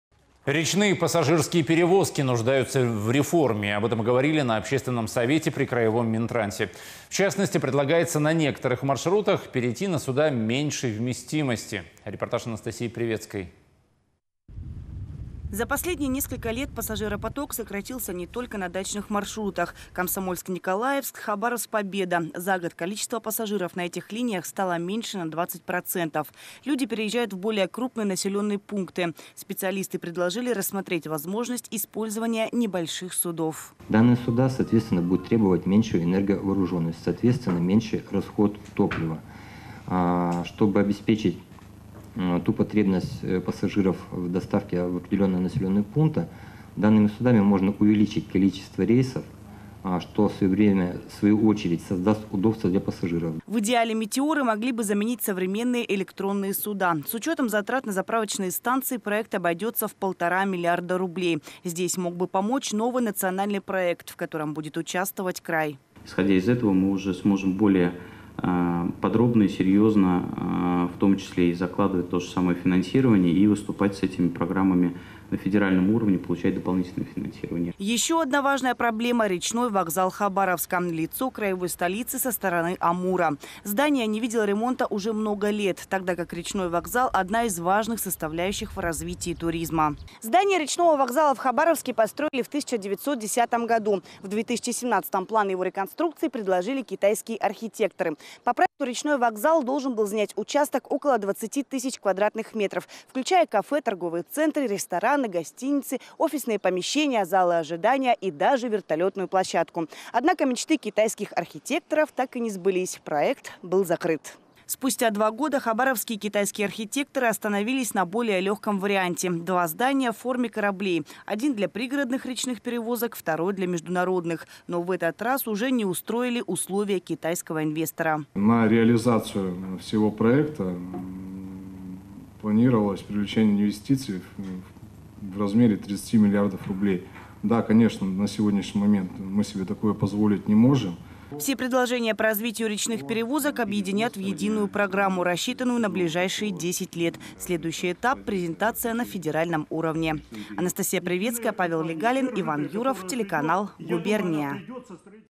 В Хабаровском крае обсуждают реформу речных перевозок. Новости. 16/06/2021
Речные пассажирские перевозки нуждаются в реформе. Об этом заявили на Общественном совете при краевом Минтрансе. В частности предлагается на некоторых маршрутах перейти на суда меньшей вместимости. Репортаж Анастасии Приветской.